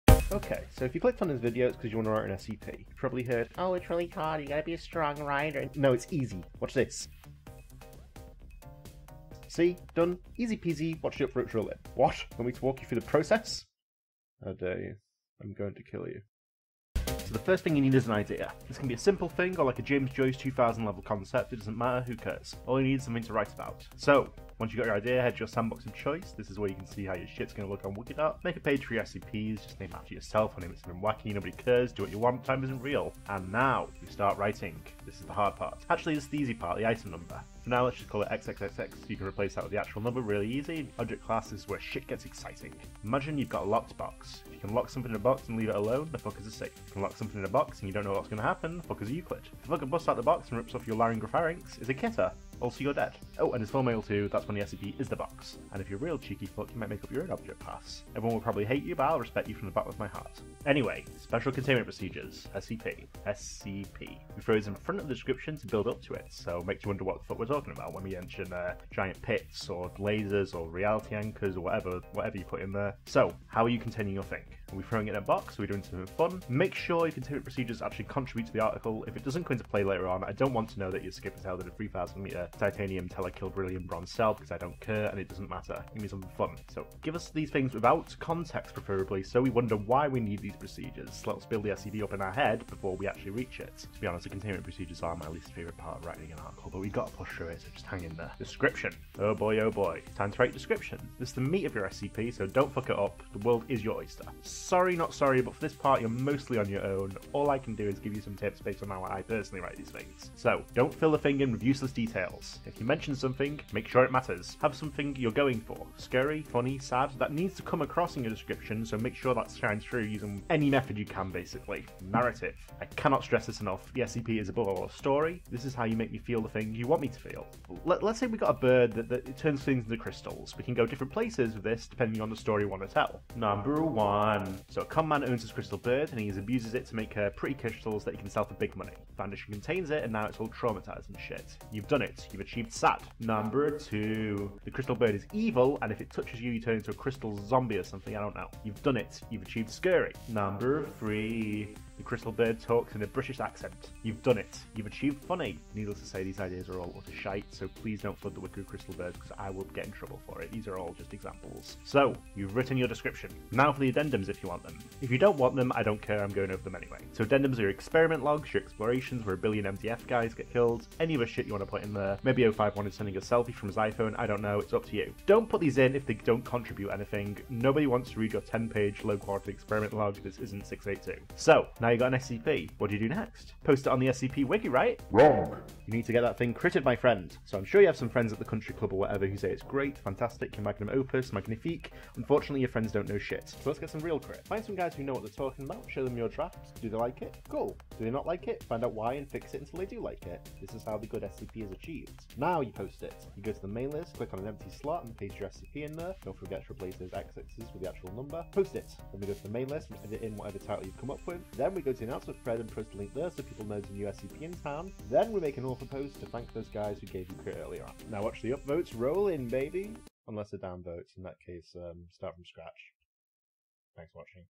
How To Write An SCP
Looking to write an SCP? Here's some (questionable) advice from ol' Tanhony!